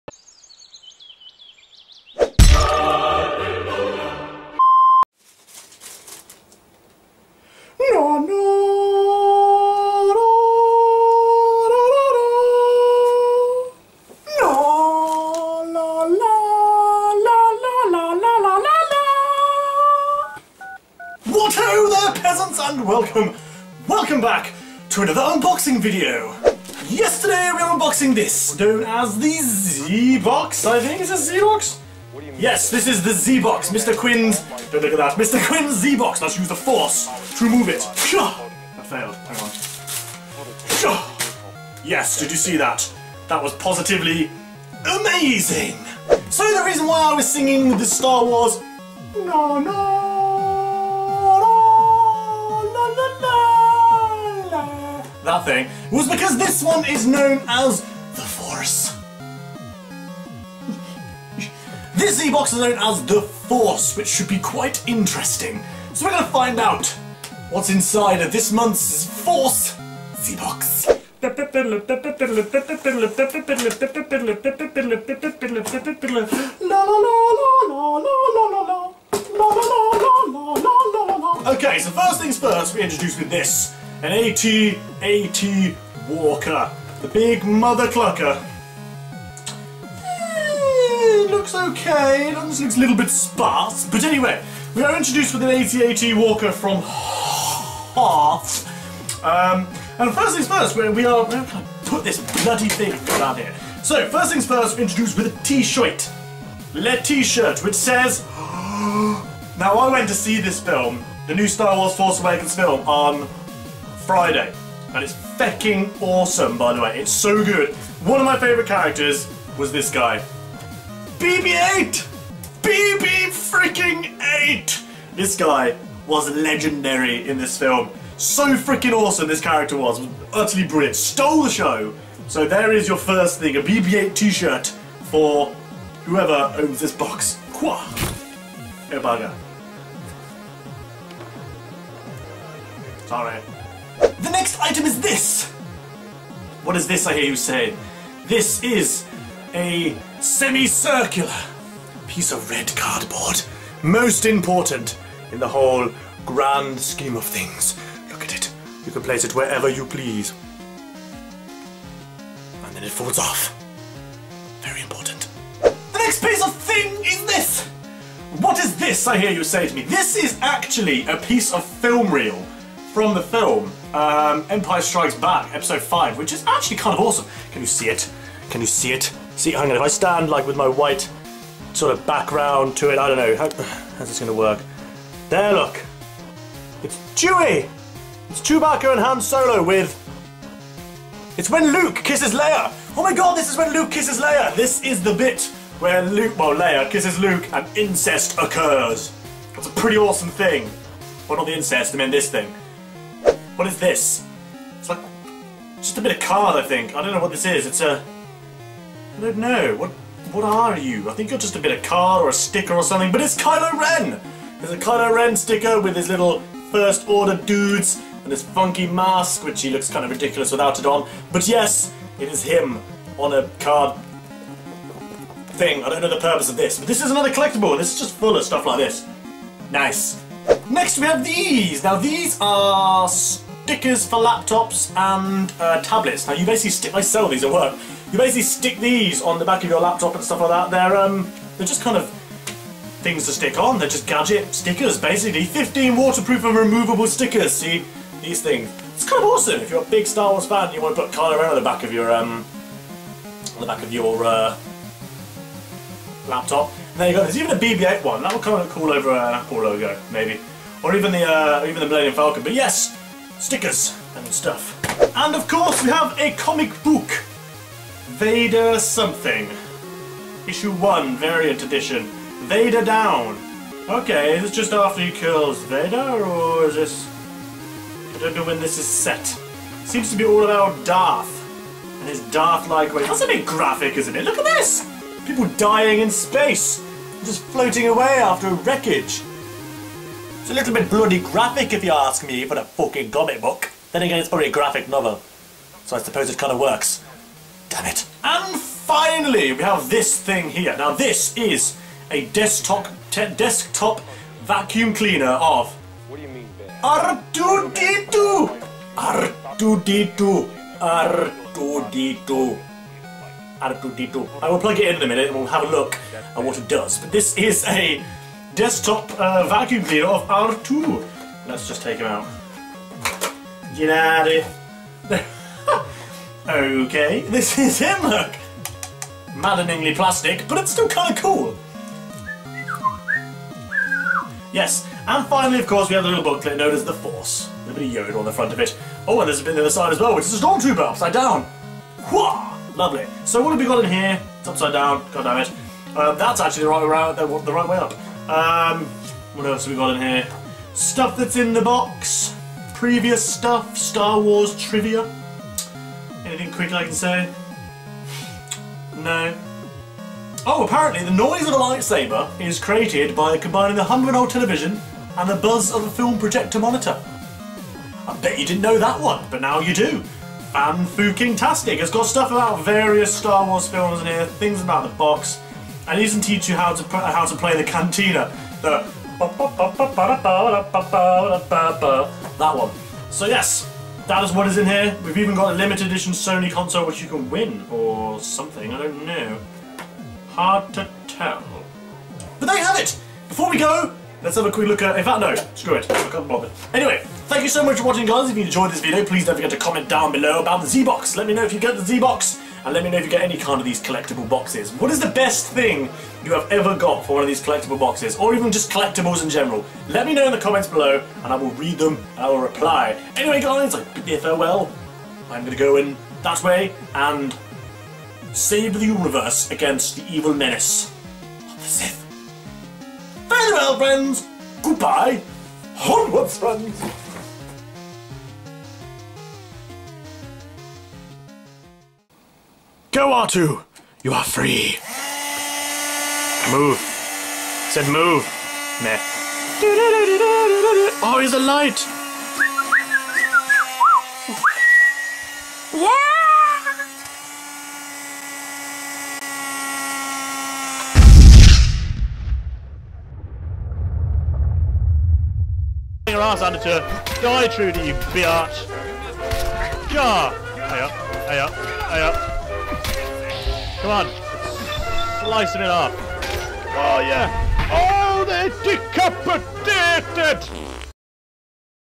The Force! Zbox December 2015 Unboxing!
Chay uses the force to unbox todays loot crate, that or its just called the force...

Razer DeathAdder Chroma
Razer Deathstalker Chroma
Razer Kraken Chroma